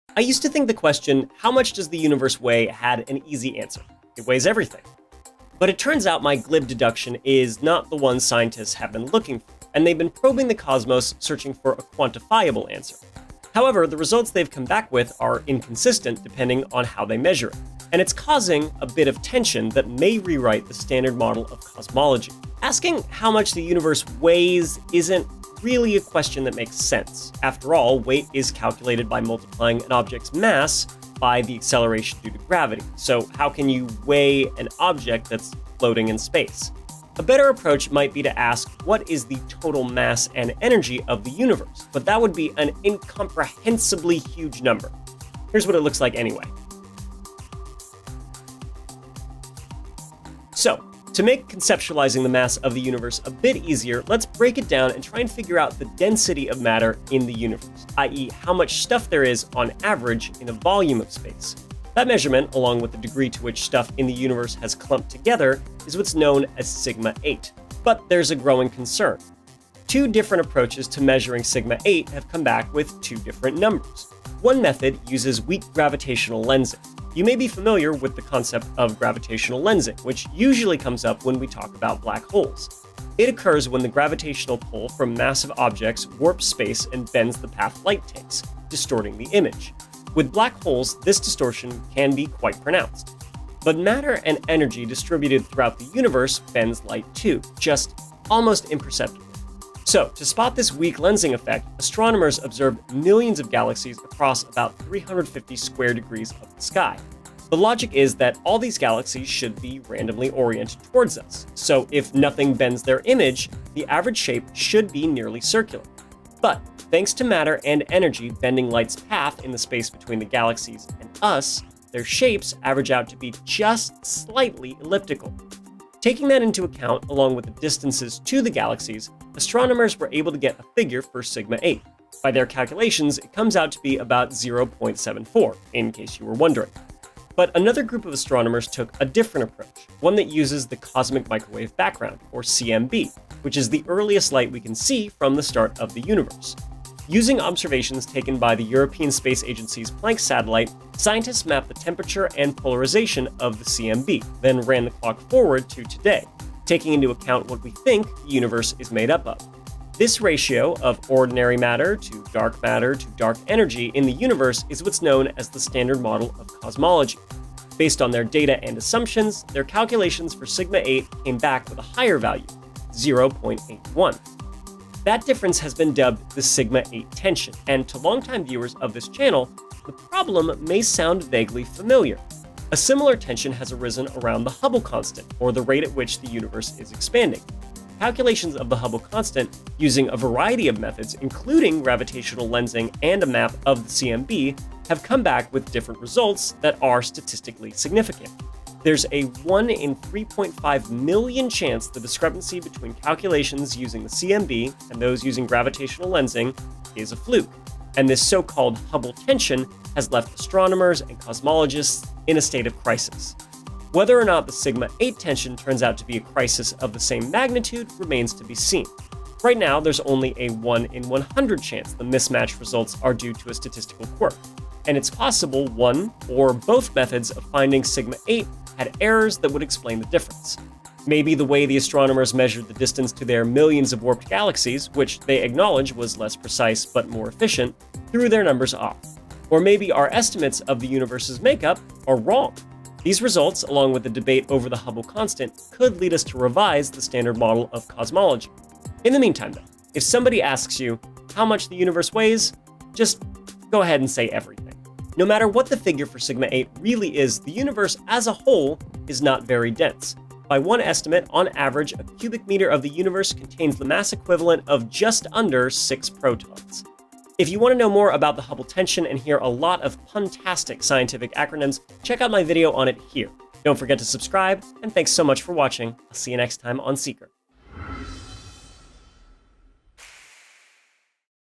Here's Why Scientists Are Still Arguing About How Much the Universe Weighs
How much does the universe weigh? As it turns out, this question does not have an easy answer.

Two different approaches to weighing the cosmos are producing different results, and the standard model of cosmology might need a rewrite.

Scientists have been probing the cosmos searching for a quantifiable answer to the question: How heavy is the universe? However, the results they’ve come back with are inconsistent depending on how they measure it, and it’s causing a bit of tension.

Find out more about the two different ways of measuring sigma-eight and what these discrepancies could mean for the future of cosmology and our understanding of the universe in this Elements.

How heavy is the universe? Conflicting answers hint at new physics.
"Two entirely different ways of "weighing" the cosmos are producing disparate results. If more precise measurements fail to resolve the discrepancy, physicists may have to revise the standard model of cosmology, our best description of the universe."

New Distance Measurements Bolster Challenge to Basic Model of Universe
"A new set of precision distance measurements made with an international collection of radio telescopes have greatly increased the likelihood that theorists need to revise the “standard model” that describes the fundamental nature of the Universe."

How Do You Weigh The Universe?
"Knowing the mass of the cosmos is central to understanding its history and evolution. While dark energy drives the universe to expand, matter tries to keep the universe from expanding."

Seeker How much does the universe weigh? As it turns out, this question does not have an easy answer.